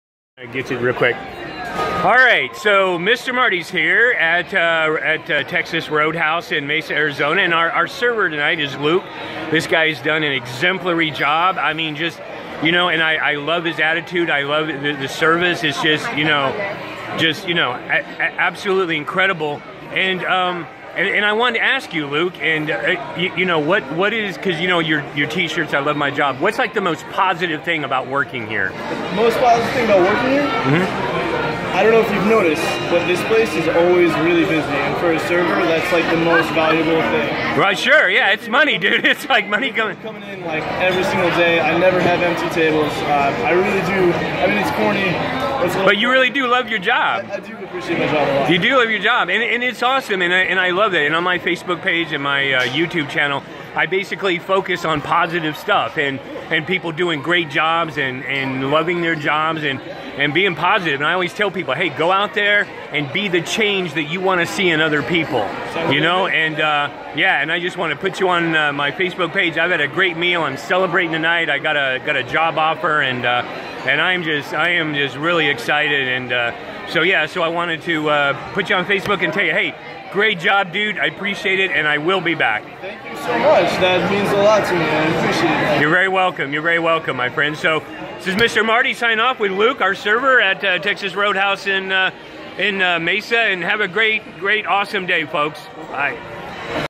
Awesome service at Texas Roadhouse!
This is Luke, our server last night at Texas Roadhouse in Mesa, AZ.   The food and the service was outstanding and I wanted ask Luke our server what is the most positive thing about his job.  His answer?  $$$$!